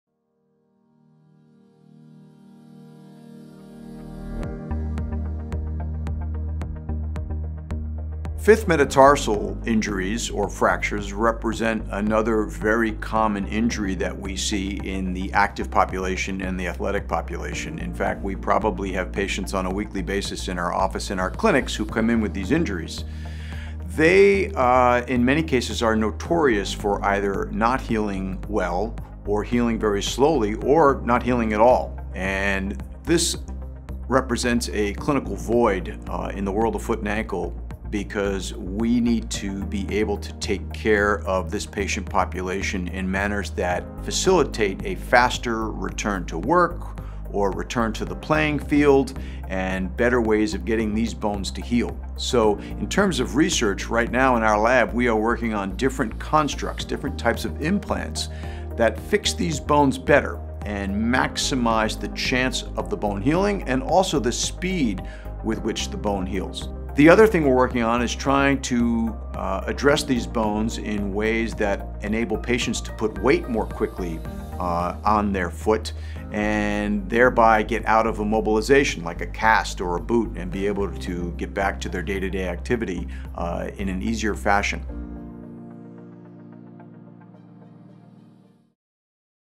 New Advances in Healing Fifth Metatarsal Injuries
Christopher W. Digiovanni, MD, chief of Foot and Ankle Service at Massachusetts General Hospital, explains how fifth metatarsal injuries burden a sizeable portion of the athletic patient population. His research team is working on better implants to increase the chance and speed of bone healing and treatment options that allow for weight-bearing sooner.